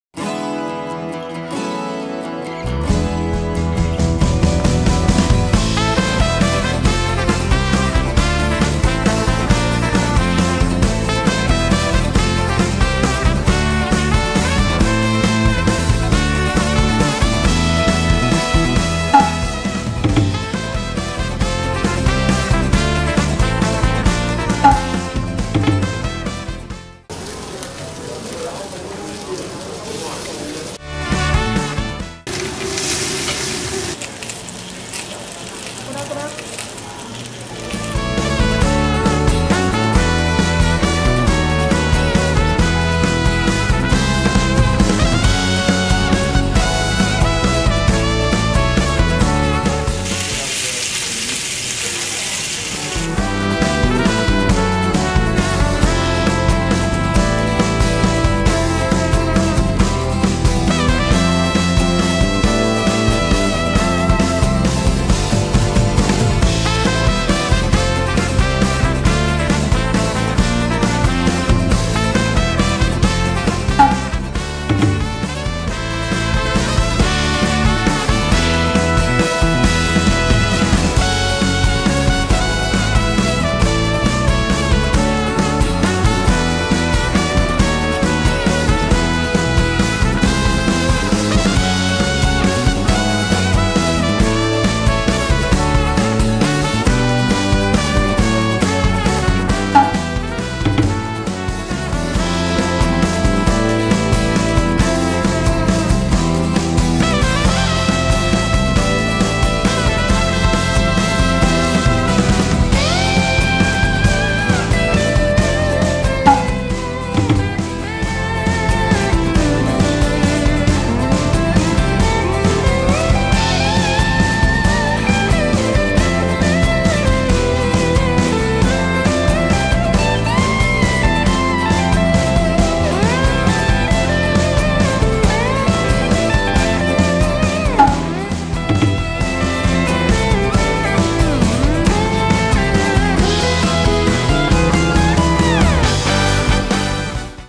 放飯新寵！中環親民價食堂
呢間地庫食堂唔單只裝修光鮮好坐，啲嘢都幾抵食。牛舌丼飯、番茄烏冬、滑蛋飯最平 $58 有交易，仲送埋糖水添。豪少少可以食而家勁 hit 的石燒牛扒，200g $76，包埋自助沙律吧、白飯同是日靚湯，呢間舖無加一㗎，中環嚟講性價比算唔錯。

【info】
滿滿食堂
地址：中環皇后大道中 58-60 號振邦大廈地庫
營業時間：星期日至四 7:30am - 9pm，星期五及六 7:30pm - 10pm
全方位緊貼《U Magazine》社交平台